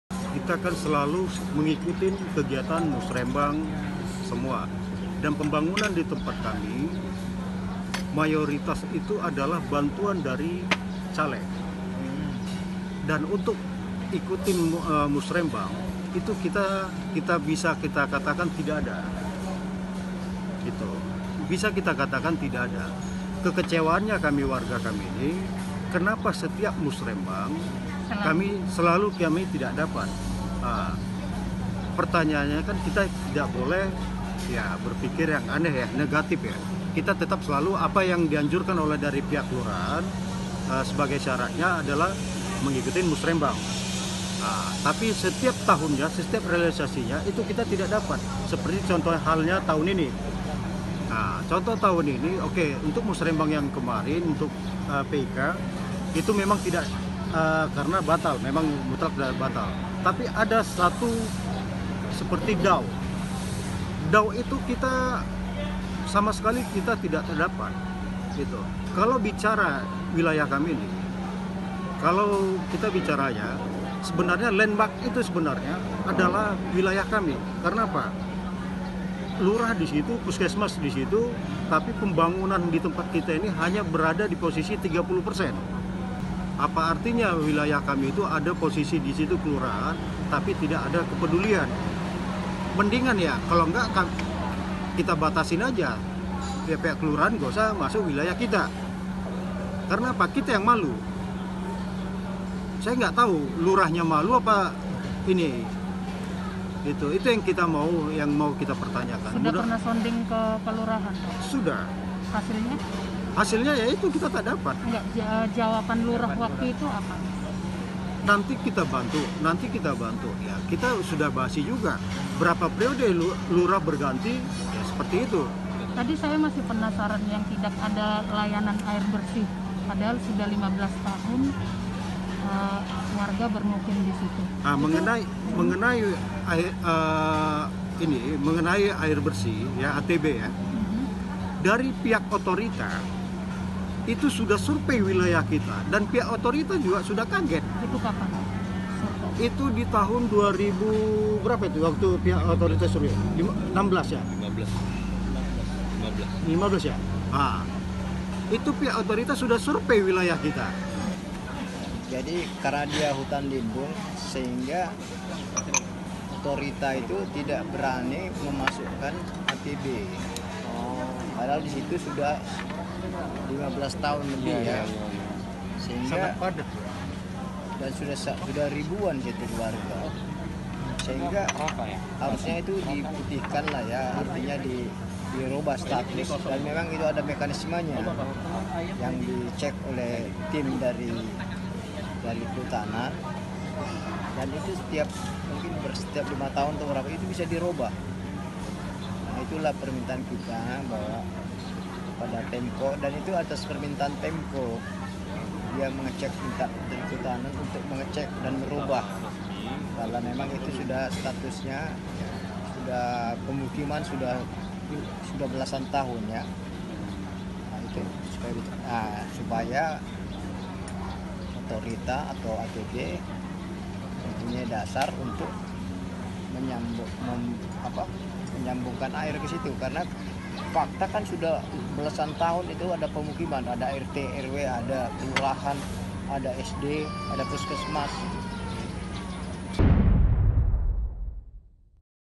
15 Tahun Warga di Pulau paling modern di Indonesia Belum Dapat Pasokan Air Bersih
Warga Bengkong Wahyu, kelurahan Tanjungbuntung, salah satu kawasan di Kecamatan Bengkong, Batam belum dialiri  air bersih hingga sekarang. 15 tahun penantian, tak kunjung terealisasi. Mereka, melalui Ketua RW dan RT berharap Pemerintah (Pemko Batam dan BP Batam) memperhatikan nasib mereka.